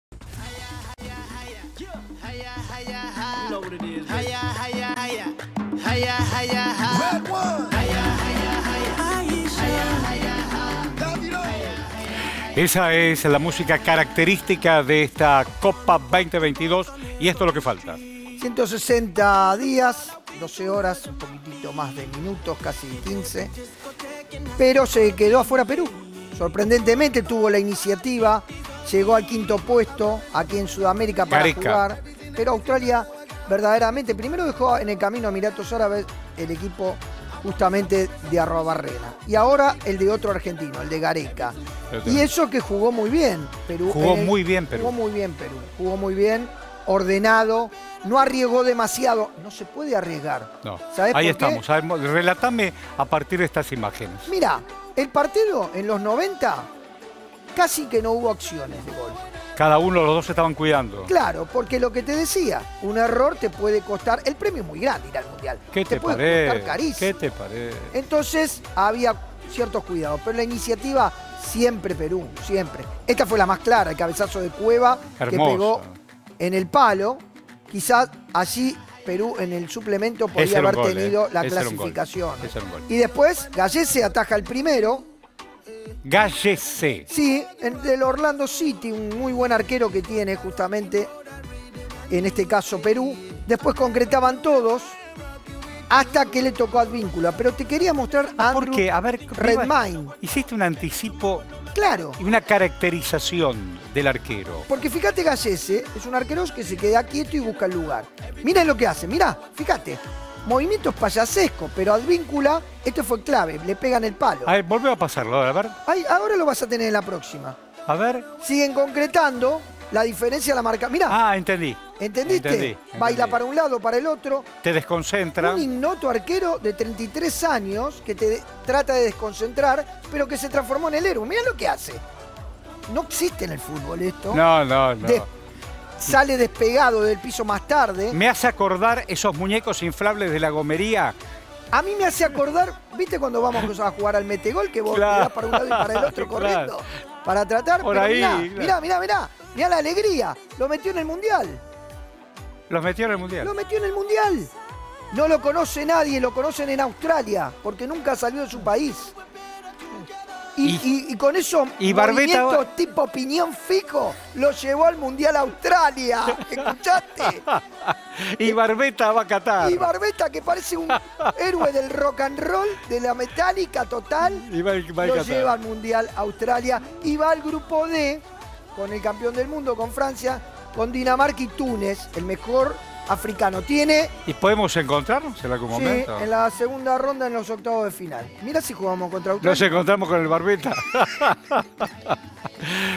Australia va al Mundial de la mano de su arquero
El arquero australiano tuvo un rol fundamental para que su país sea quien clasifique al Mundial. Perú no pudo obtener el último lugar para Qatar.